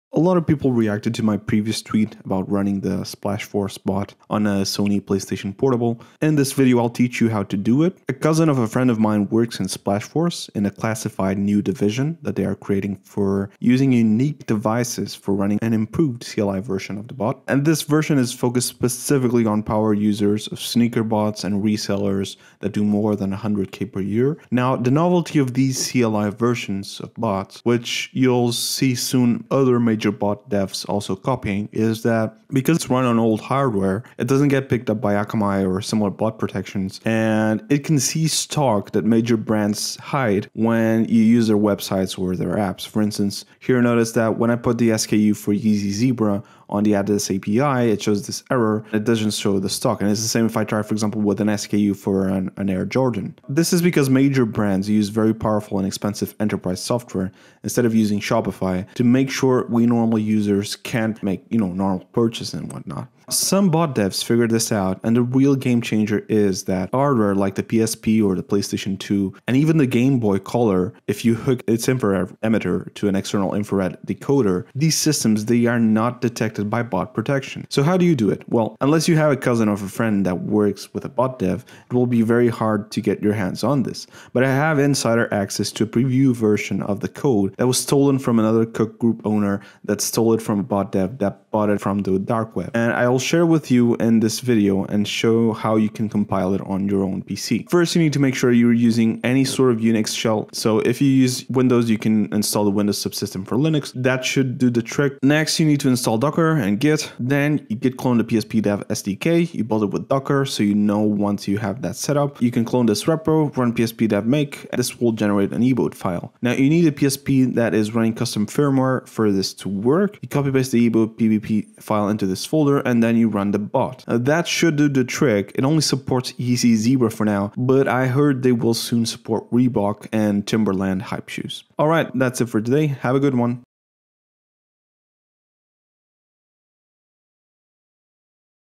Splashforce on the PSP - Tutorial
Tutorial on running the Splashforce bot on the Sony PSP.

If you are interested in knowing more about homebrew development for the PSP, you can check out these links:

(FYI this video is a parody :D)